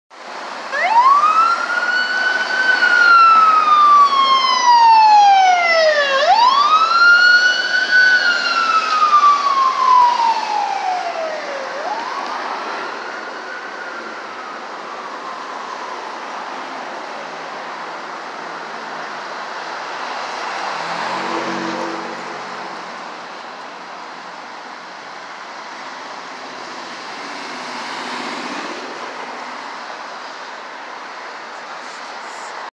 LAFD Rescue 87 Responding
LAFD Rescue 87 responding to a medical call in 18's district

Units Due: E18, RA87

Granada Hills, Los Angeles, California

Filmed: Friday, October 14, 2022